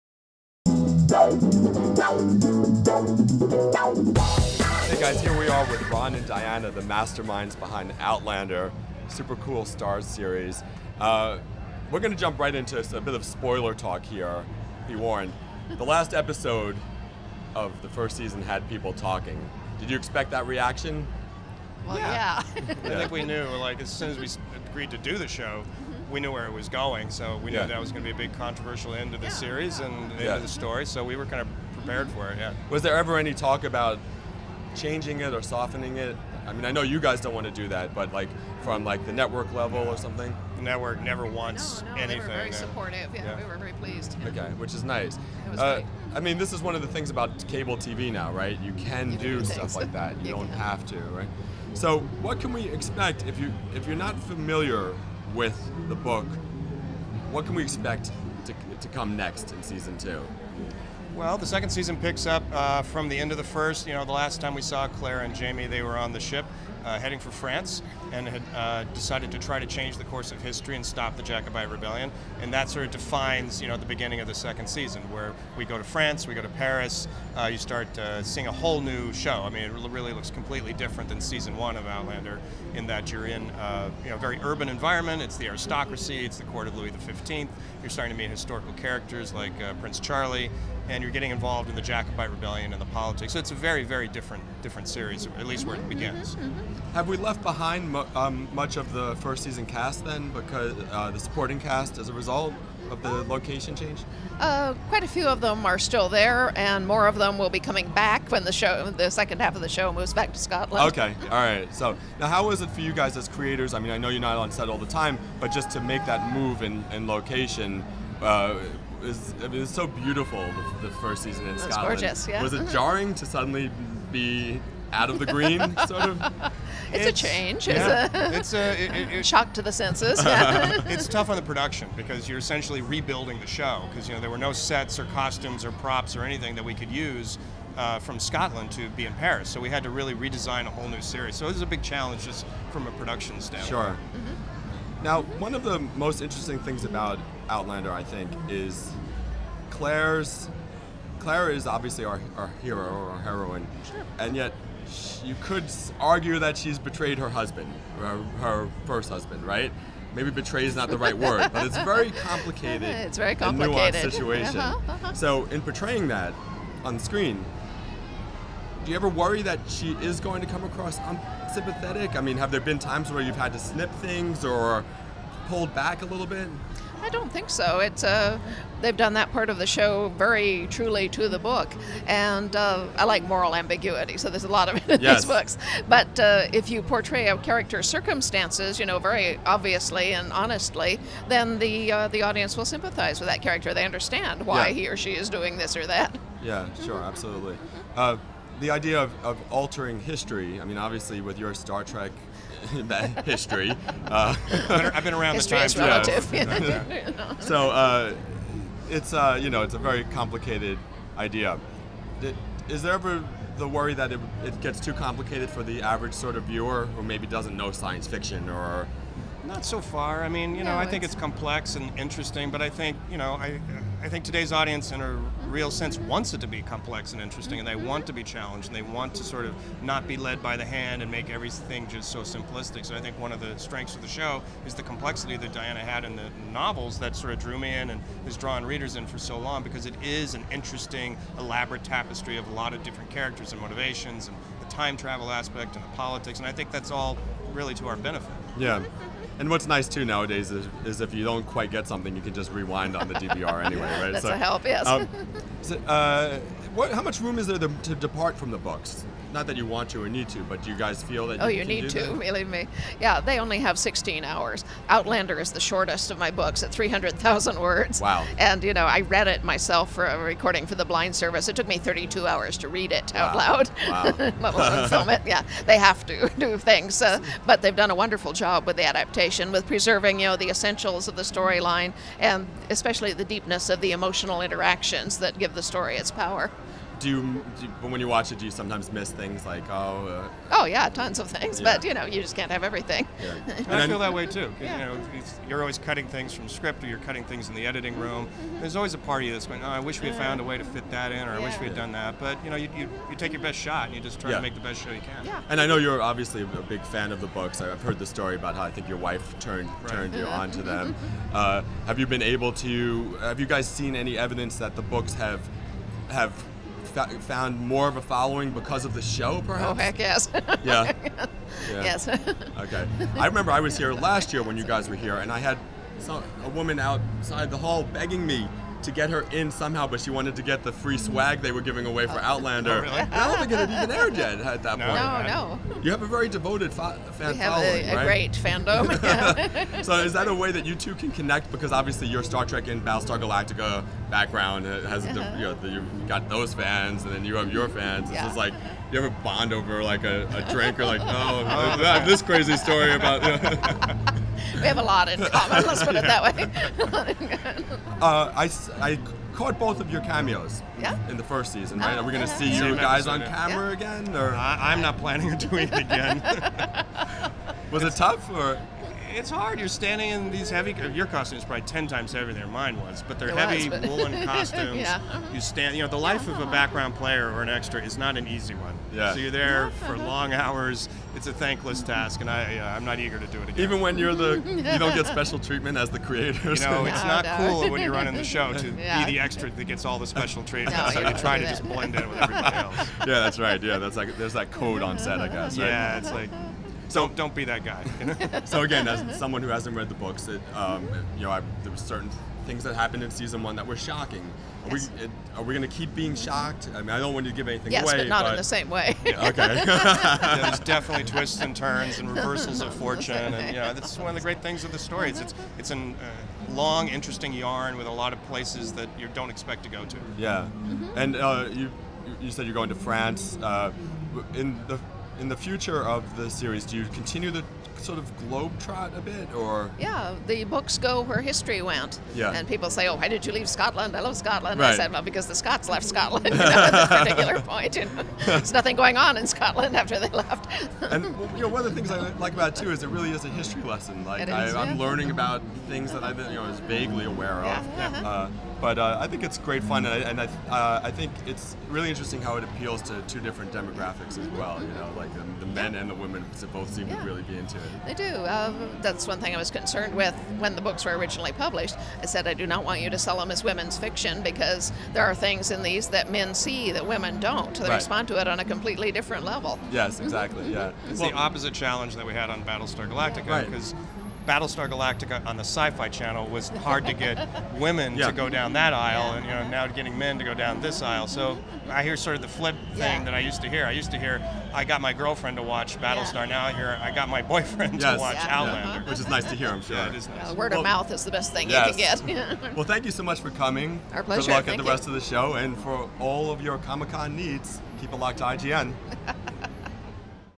Outlander: Diana Gabaldon, Ronald D. Moore Season 2 Interview - Comic-Con 2015
The show's Executive Producer and book author on what to expect from the Starz hit in Season 2.